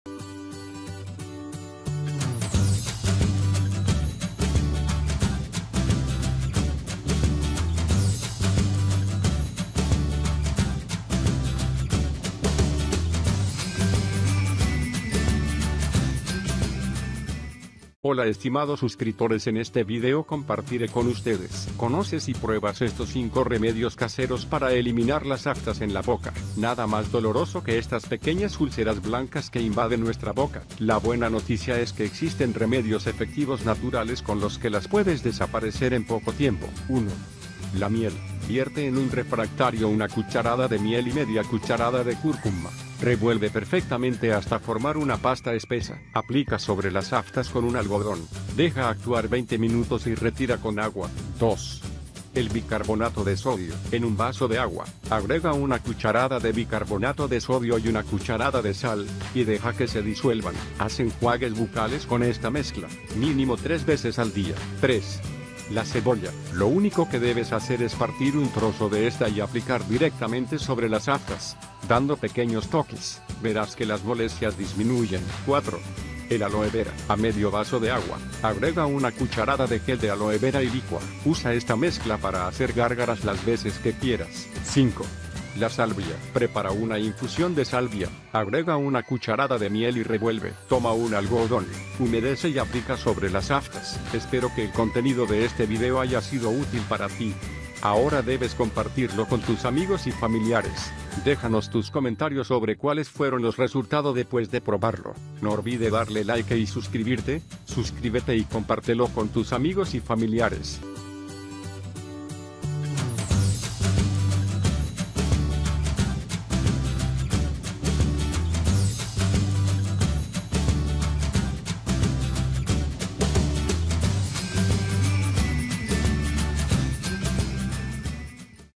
Conoces y pruebas estos 5 remedios caseros para eliminar las aftas en la boca
Nada más doloroso que estas pequeñas úlceras blancas que invaden nuestra boca… La buena noticia es que existen remedios efectivos naturales con los que las puedes desaparecer en poco tiempo.

1. la Miel.

Vierte en un refractario una cucharada de miel y media cucharada de cúrcuma, revuelve perfectamente hasta formar una pasta espesa. Aplica sobre las aftas con un algodón, deja actuar 20 minutos y retira con agua.

2. el Bicarbonato de sodio.

En un vaso de agua, agrega una cucharada de bicarbonato de sodio y una cucharada de sal, y deja que se disuelvan. Haz enjuagues bucales con esta mezcla, mínimo tres veces al día.

3. la Cebolla.

Lo único que debes hacer es partir un trozo de esta y aplicar directamente sobre las aftas, dando pequeños toques. Verás que las molestias disminuyen.

4. el Aloe vera.

A medio vaso de agua, agrega una cucharada de gel de aloe vera y licua. Usa esta mezcla para hacer gárgaras las veces que quieras.

5. la Salvia.

Prepara una infusión de salvia, agrega una cucharada de miel y revuelve. Toma un algódón, humedece y aplica sobre las aftas.